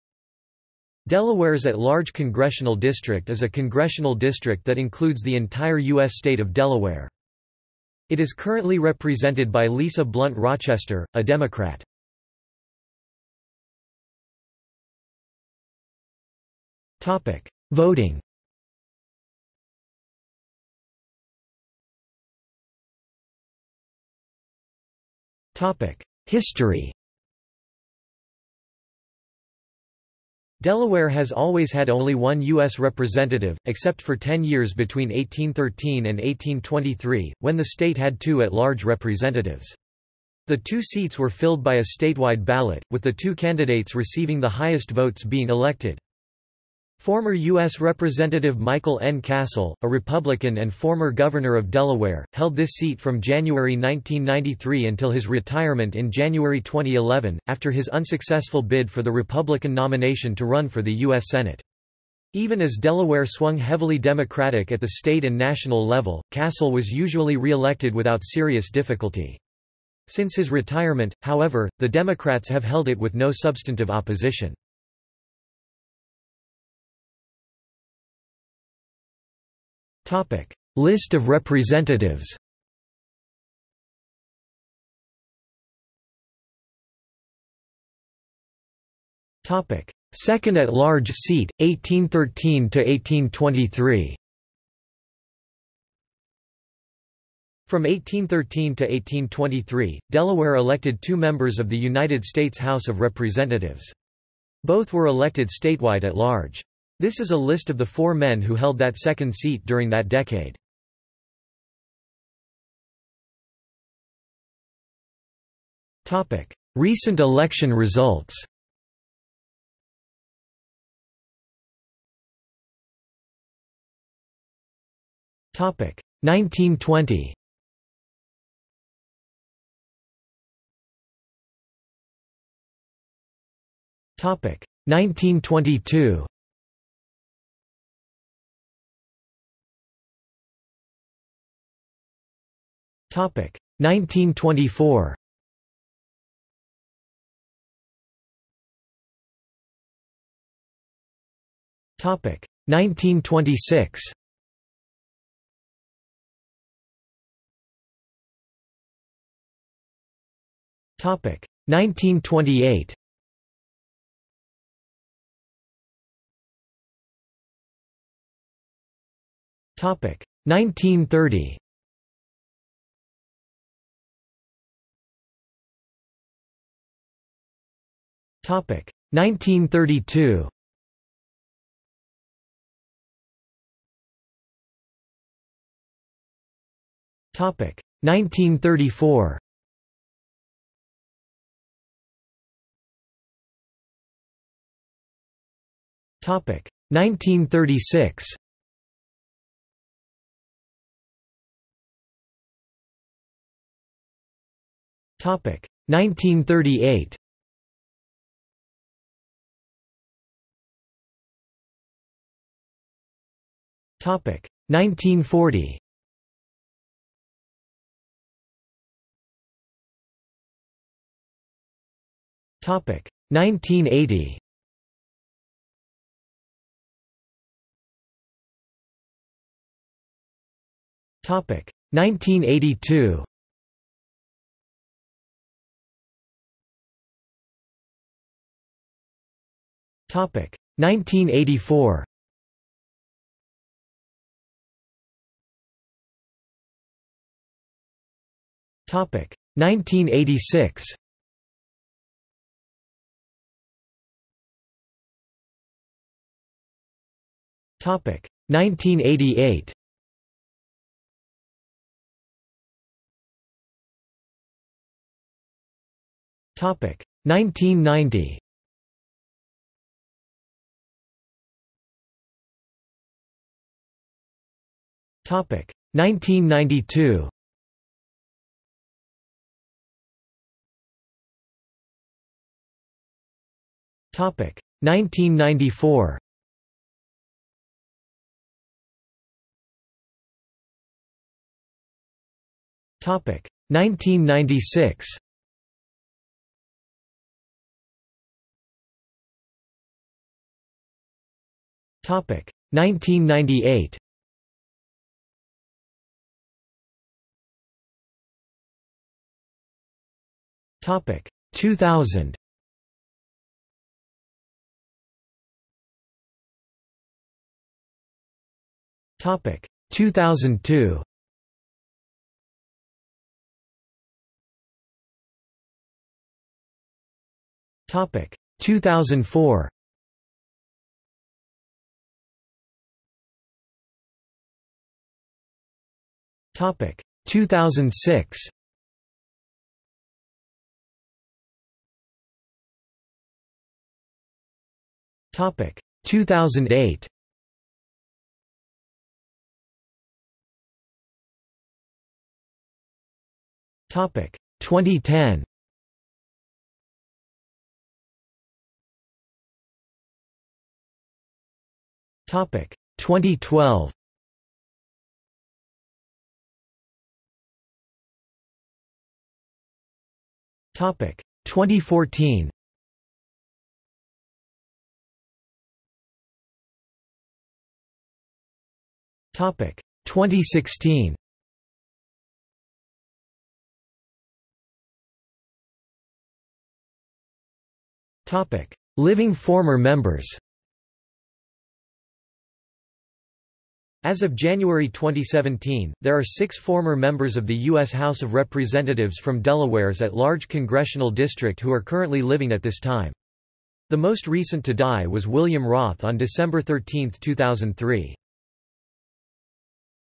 Delaware's at-large congressional district | Wikipedia audio article
This is an audio version of the Wikipedia Article:
Delaware's at-large congressional district

00:00:13 1 Voting
00:00:21 2 History
00:01:16 3 List of representatives
00:01:26 3.1 Second at-large seat: 1813–1823
00:01:52 4 Recent election results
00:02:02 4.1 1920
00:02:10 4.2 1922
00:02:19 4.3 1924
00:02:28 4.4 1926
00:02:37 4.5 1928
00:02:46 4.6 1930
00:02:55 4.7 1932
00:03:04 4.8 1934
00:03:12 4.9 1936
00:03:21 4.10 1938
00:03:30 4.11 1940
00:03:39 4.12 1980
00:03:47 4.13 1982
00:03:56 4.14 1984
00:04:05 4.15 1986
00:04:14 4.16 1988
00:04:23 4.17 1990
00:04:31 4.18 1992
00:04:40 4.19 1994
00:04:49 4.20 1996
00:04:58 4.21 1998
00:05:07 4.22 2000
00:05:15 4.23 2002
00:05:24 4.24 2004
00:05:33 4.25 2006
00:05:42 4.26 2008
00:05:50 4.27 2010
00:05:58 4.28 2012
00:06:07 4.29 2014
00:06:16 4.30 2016
00:06:24 5 Living former Members

- Socrates

SUMMARY
Delaware's at-large congressional district is a congressional district that includes the entire U.S. state of Delaware.
It is currently represented by Lisa Blunt Rochester, a Democrat.